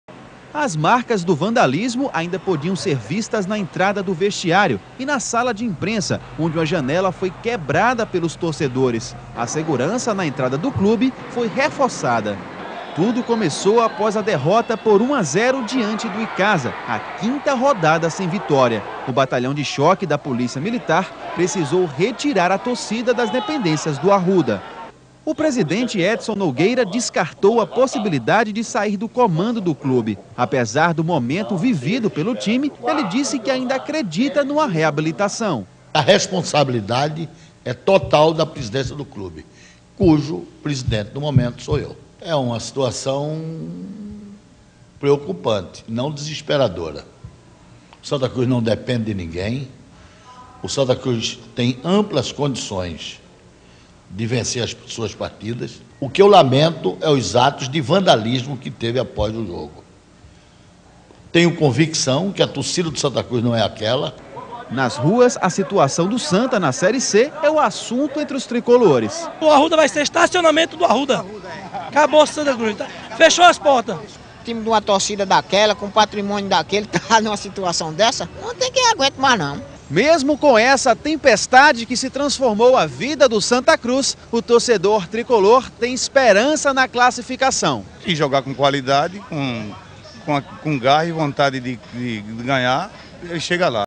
Santa Cruz 0 x 1 Icasa CE 2008
Reportagem mostra a crise vivida pelo Santa Cruz na Série C de 2008, o clube acabaria rebaixado para Série D de 2009.